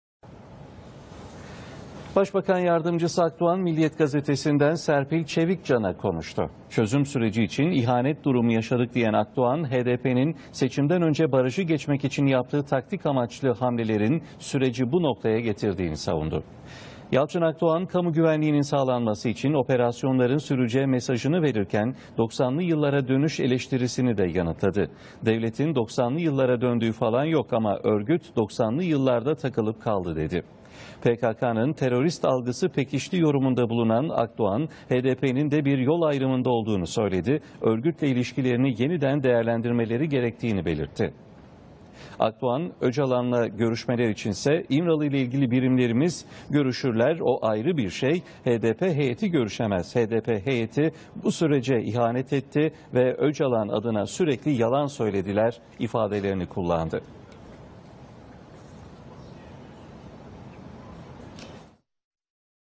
Akdoğan Süreci İstismar Edip İhanet Ettiler, Aktörler Değişebilir
Başbakan Yardımcı Akdoğan, "HDP terörle mesafeli olmalı artık yol ayrımında. Süreç ve Öcalan istismar edildi. Aktörler yeniden değerlendirilebilir." dedi.